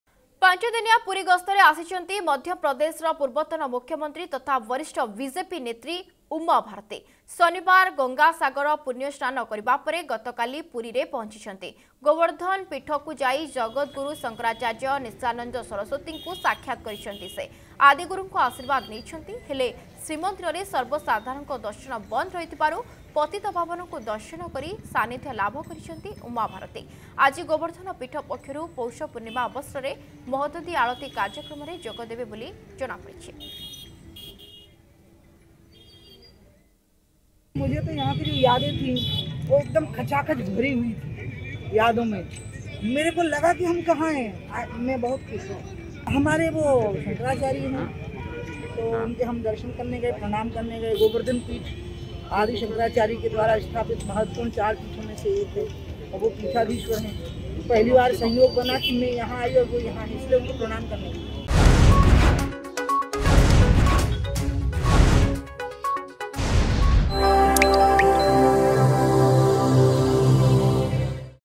ପାଞ୍ଚଦିନିଆ ପୁରୀ ଗସ୍ତରେ ବିଜେପି ନେତ୍ରୀ ଉମାଭାରତୀ
ପାଞ୍ଚଦିନିଆ ପୁରୀ ଗସ୍ତରେ ଆସିଛନ୍ତି ମଧ୍ୟପ୍ରଦେଶ ପୂର୍ବତନ ମୁଖ୍ୟମନ୍ତ୍ରୀ ତଥା ବରିଷ୍ଠ ବିଜେପି ନେତ୍ରୀ ଉମାଭାରତୀ।